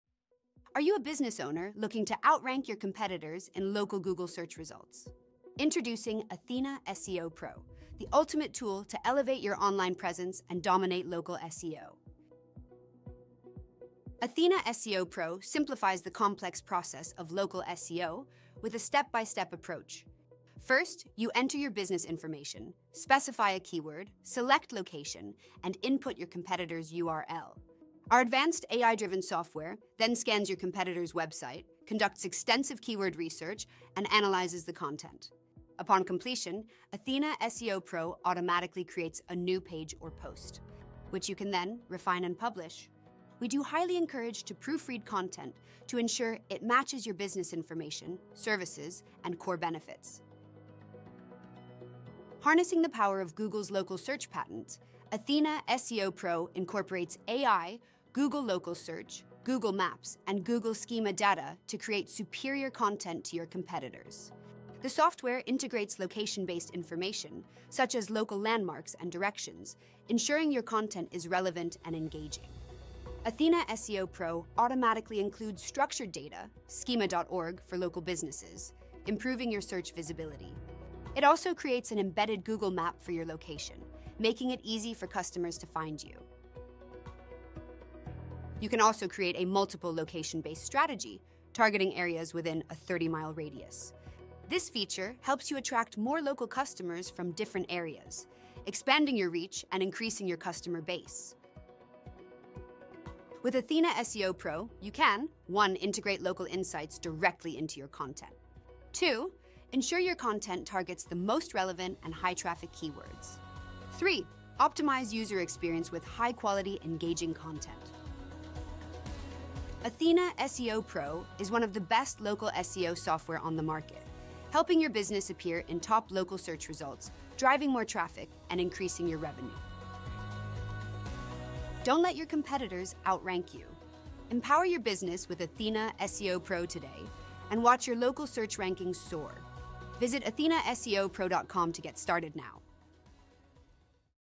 How To Rank #1 on GOOGLE in 24 HOURS
Hey, what’s going on everyone. I’m excited to introduce my new software that my team and I have been developing for the last six months Athena SEO PRO!

Athena, SEO pro is a WordPress plug-in that is integrated with AI built to help people at all skill levels of web design, rank their businesses website to the top of Google and guarantee first place. We built in a tool that analyzes your competitor based on their place on Google for specific keywords, breaks apart their website rewrite the content for your website and your business based on that keyword and guarantees you first place!

I will be making more videos on this to show you guys however revolutionary this actually is.